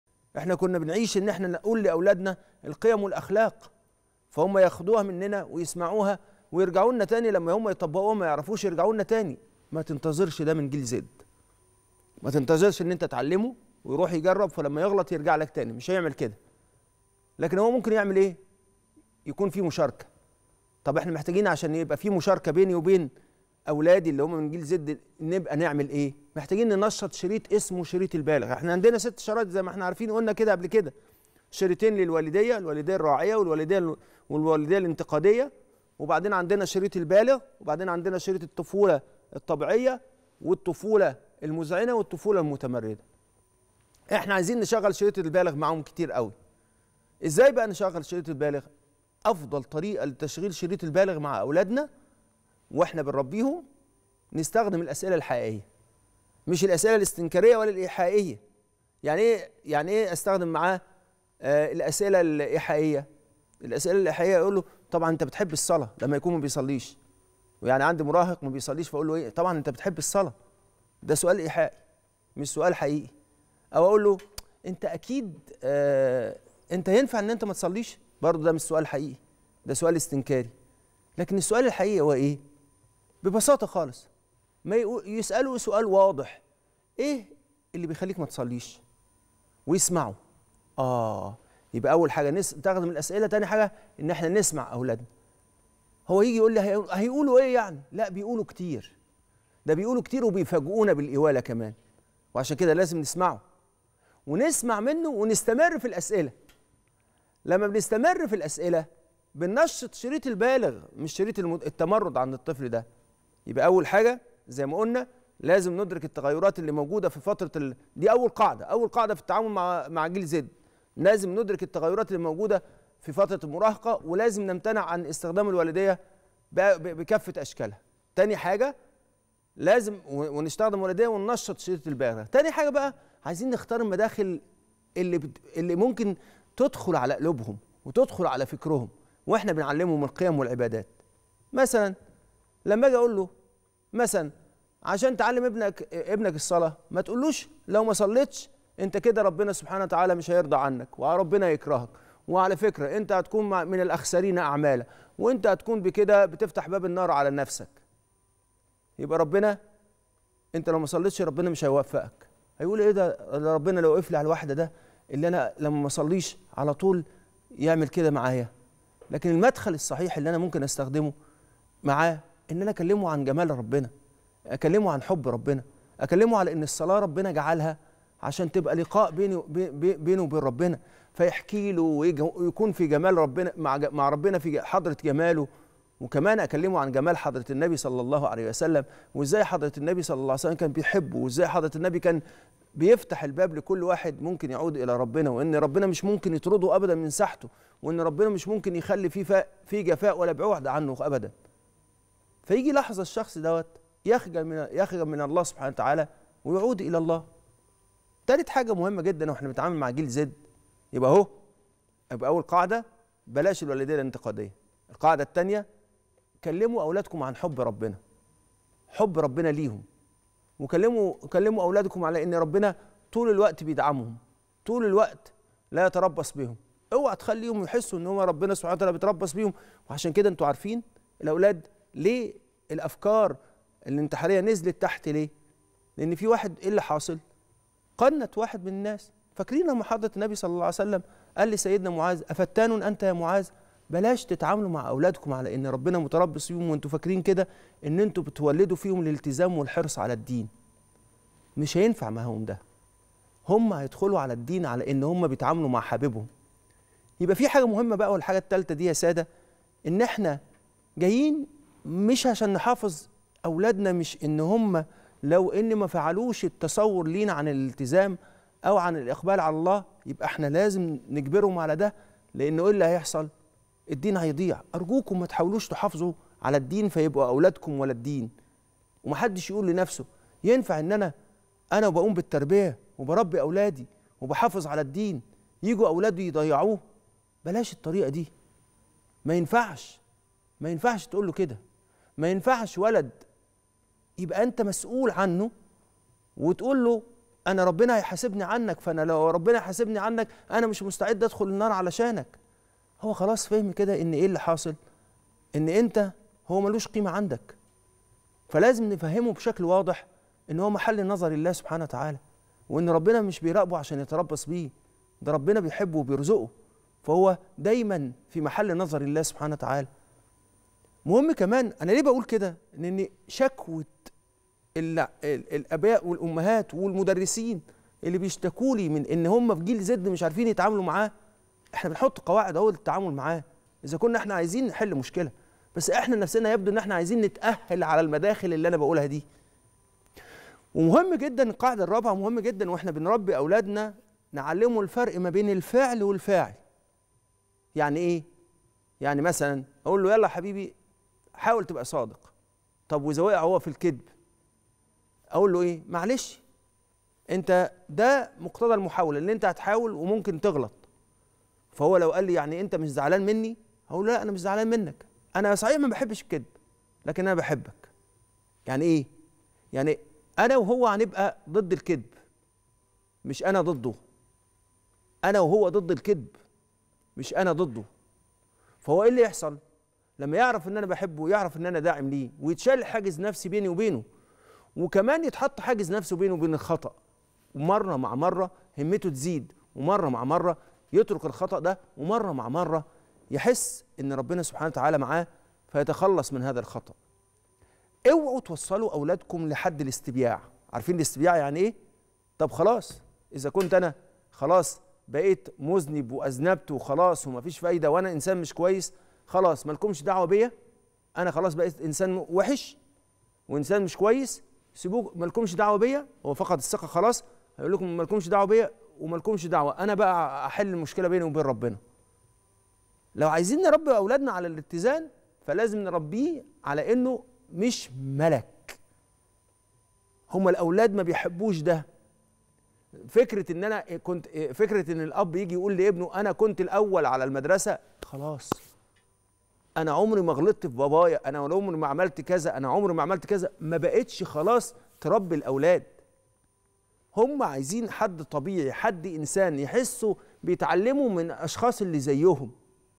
تعاملوا مع أولادكم كبالغين وليس كمراهقين.. د. عمرو الورداني يكشف أفكار "جيل زد" وكيفية التعامل معها
ولا تعسروا | تعاملوا مع أولادكم كبالغين وليس كمراهقين.. د. عمرو الورداني يكشف أهم أفكار "جيل زد" وكيفية التعامل معها